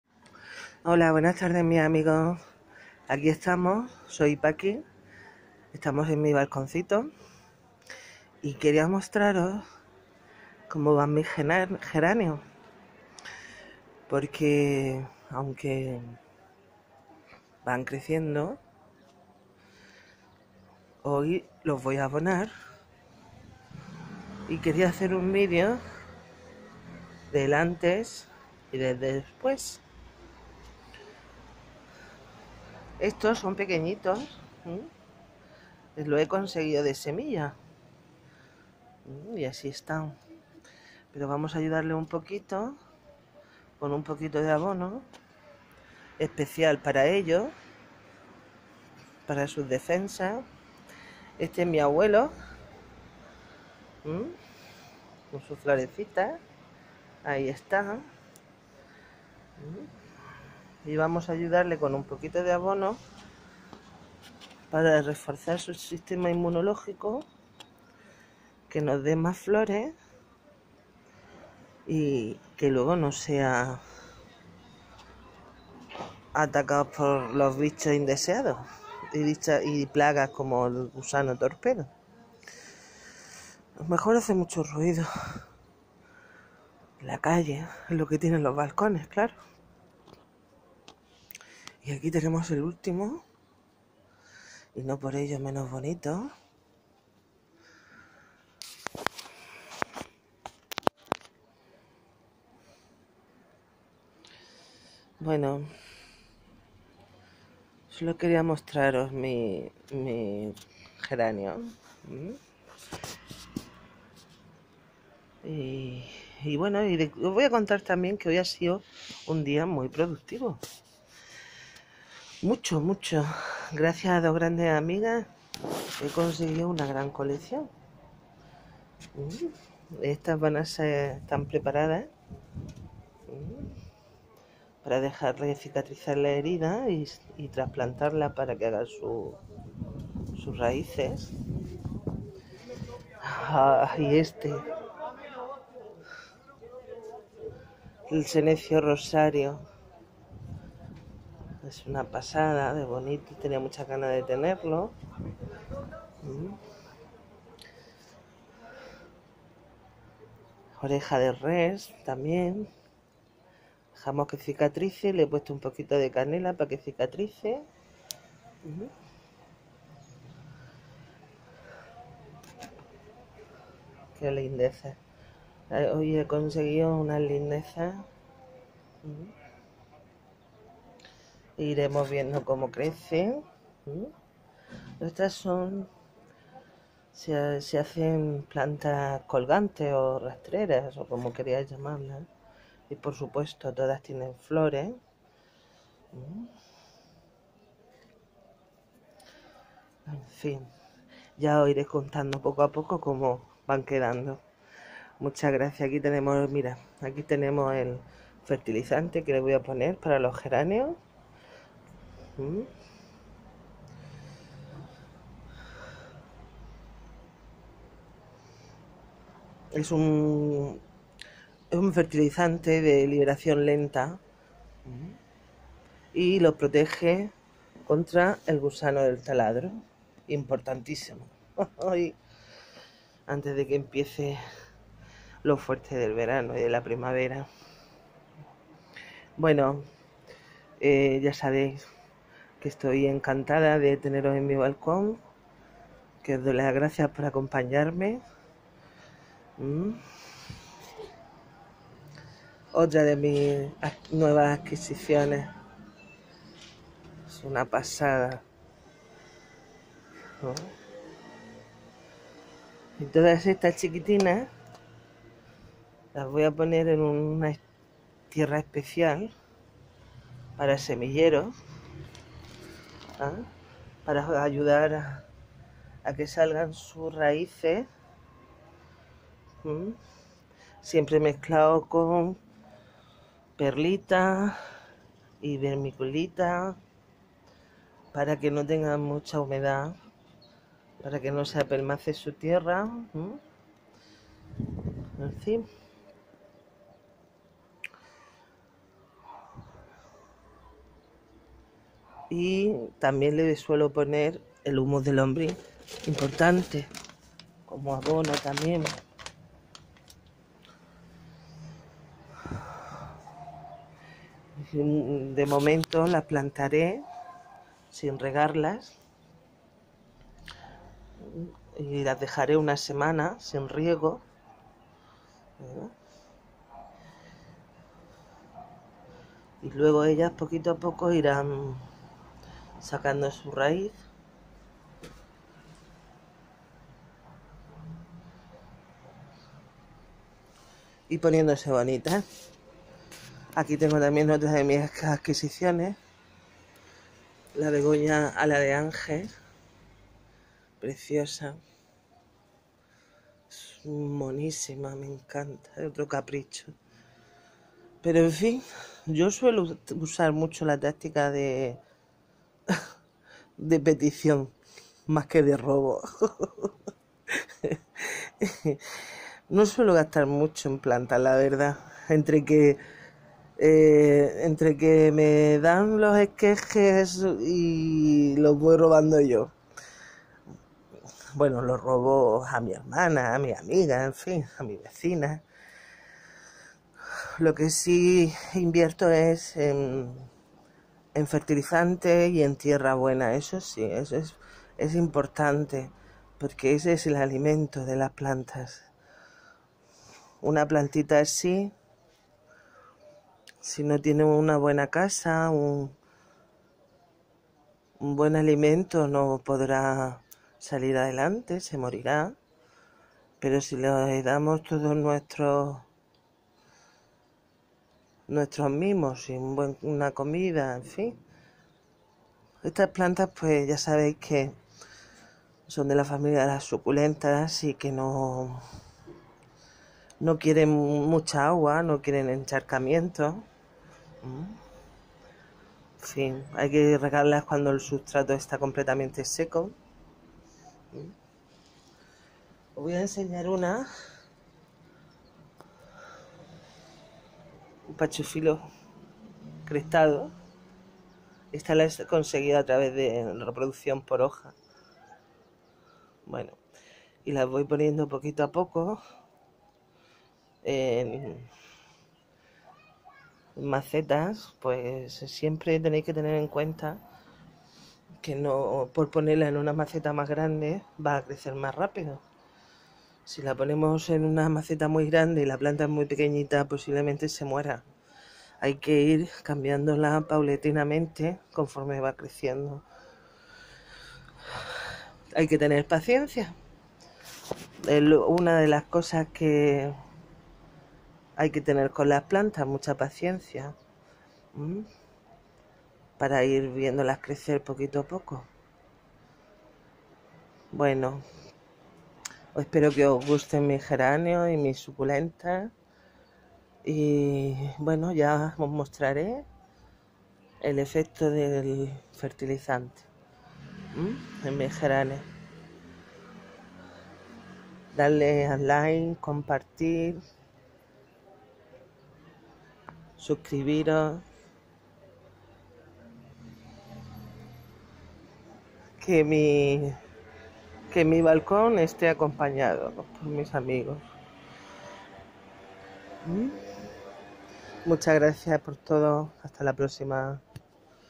Mis geranios necesitarán fertilizantes.
Hola, amigos jardineros🤗🤗🤗

Con éste vídeo quiero mostrar que las plantas dan muchas alegrías cuando las vemos crecer felices, responden contentas a nuestros cuidados ofreciéndonos su belleza 💫💫pero también sufrimos con ellas cuando las vemos
 enfermas,😓😓 aunque le demos los mejores cuidados.

 🗣️🗣️🗣️Mensaje para principiantes, no sois asesinos de plantas. Las plantas también tienen su ciclo vital.

No dudéis en poner una planta en vuestras vidas.👏👏👏

comparto un vídeo donde podéis ver un Pequeño tour por los geranios de mi balcón antes de fertilizarlos.